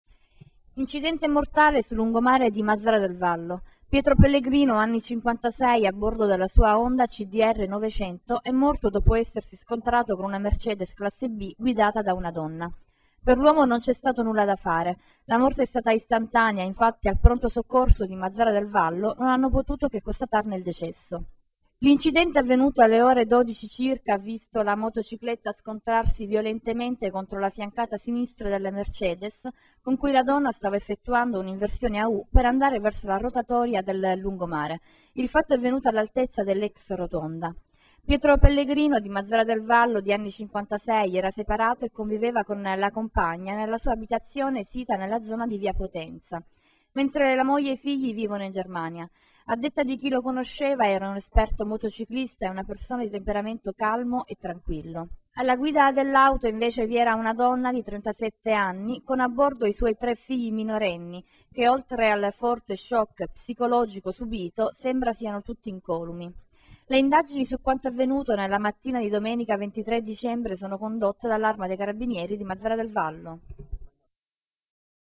2012-12-23 Incidente mortale a lungomare Mazara del Vallo
Incidente mortale sul lungomare Mazara del Vallo. Servizio di Piera Pipitone